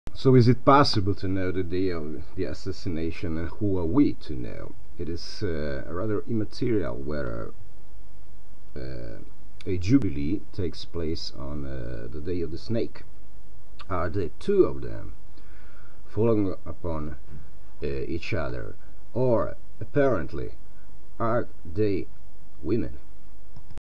Astrologer to Prevent Obama Assassination
prevents Obama's assassination by publishing the hazardous dates.